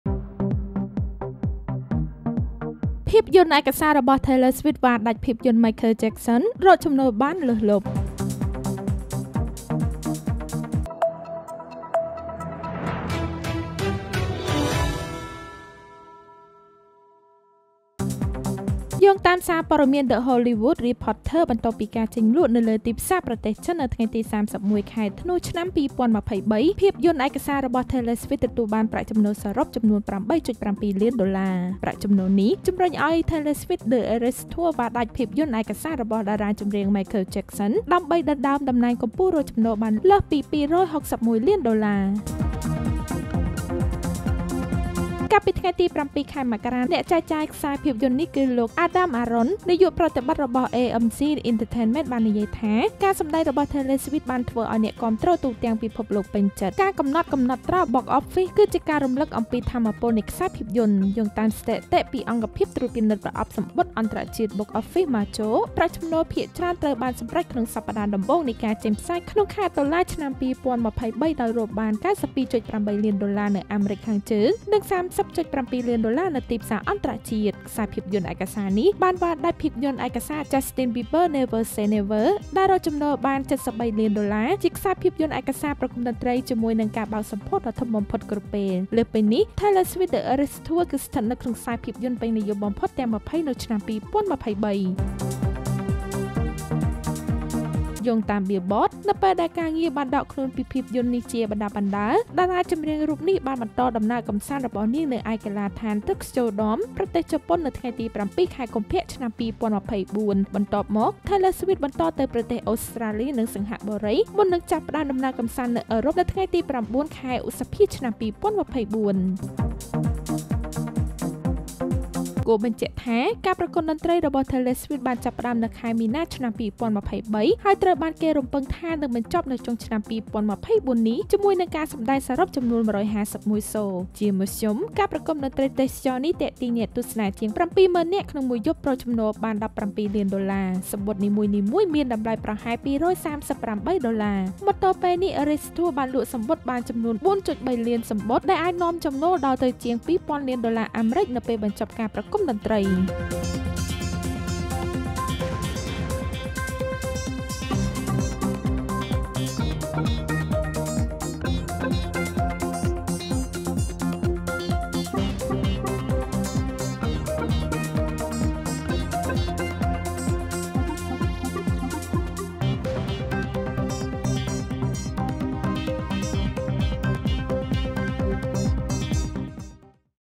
ភាពយន្តឯកសាររបស់ Taylor Swift វ៉ាដាច់ភាពយន្ត Michael Jackson រកចំណូលបានលើសលុប
យោងតាមសារព័ត៌មាន The Hollywood Reporter បន្ទាប់ពីការចេញលក់នៅលើទីផ្សារប្រទេសចិននៅថ្ងៃទី៣១ ខែធ្នូ ឆ្នាំ២០២៣ ភាពយន្តឯកសាររបស់ Taylor Swift ទទួលបានប្រាក់ចំណូលសរុបចំនួន ៨.៧ លានដុល្លារ។ ប្រាក់ចំណូលនេះជំរុញឱ្យ Taylor Swift: The Eras Tour វ៉ាដាច់ភាពយន្តឯកសាររបស់តារាចម្រៀង Michael Jackson ដើម្បីដណ្តើមតំណែងកំពូលរកចំណូលបានលើស២៦១ លានដុល្លារ។